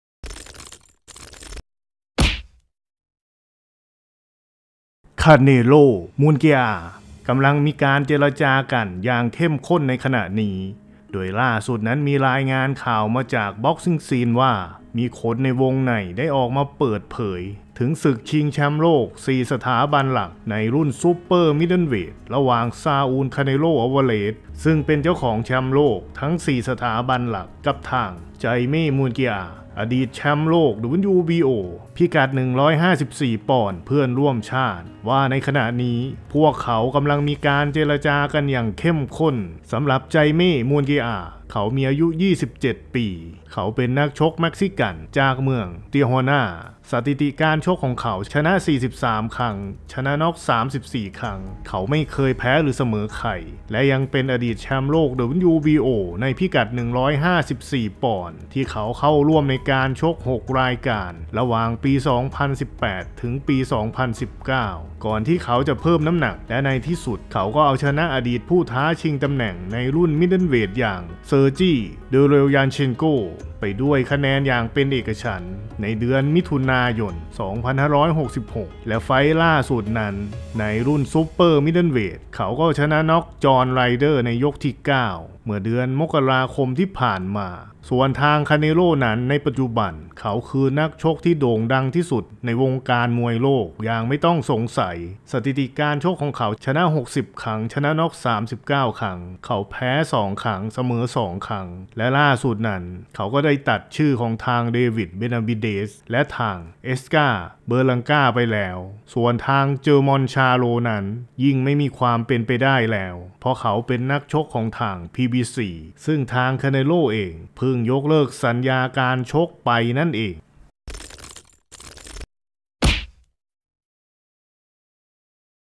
ล่าสุด! ศึกสายเลือดเม็กซิกัน "คาเนโล่ - มุนเกียอา" มีการเจรจากันอย่างเข้มข้น ในขณะนี้
คลิปนำเสนอข่าว ล่าสุด! ศึกสายเลือด "คาเนโล่ - มุนเกียอา" มีการเจรจากันอย่างเข้มข้น ในขณะนี้